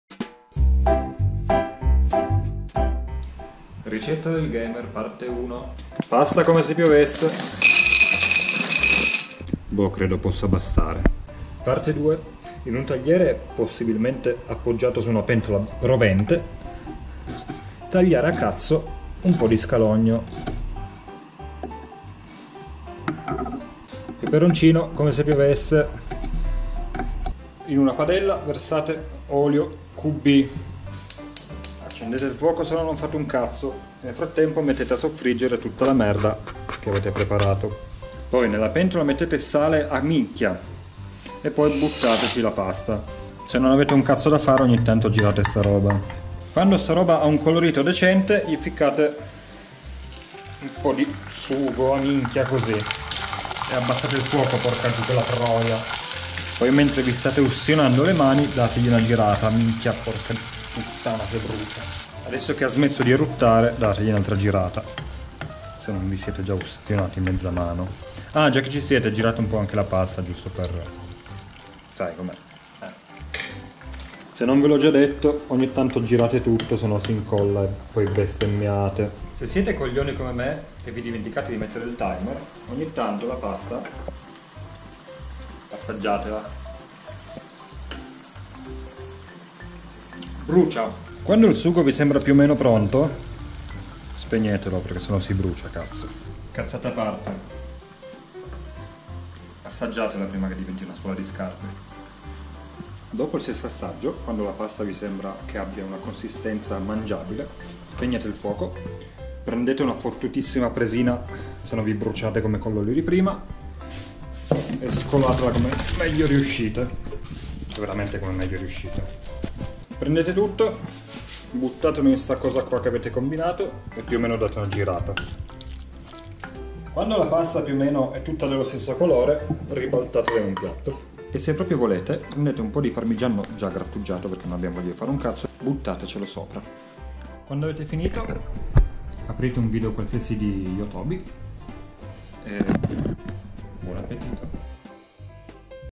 Cucina da Gamer - Ep 1 - La Pasta del Gamer
Ebbene si. Anche noi Gamer cuciniamo.
Ed ecco a voi il primo episodio di Cucina da Gamer.
Oggi prepareremo insieme la pasta del gamer.
Seguiteci per altre ricette.

E buona permanenza tra i Pessimi Elementi!

Vuoi comprare giochi e abbonamenti scontati e nel mentre supportarci?

Grazie della visita!